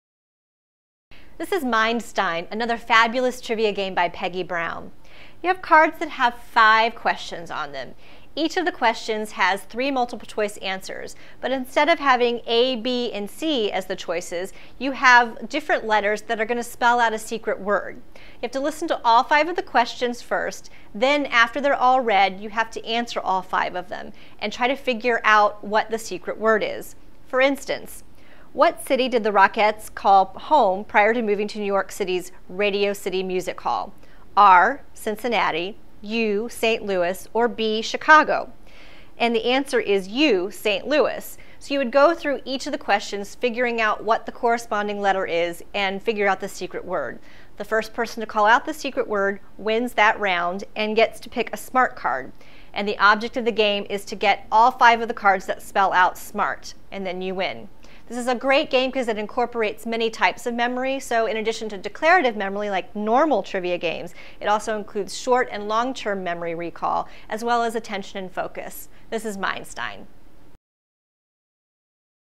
Mindstein
Marbles BrainCoach Paulette explains the Marbles exclusive trivia game Mindstein created by game designer Peggy Brown.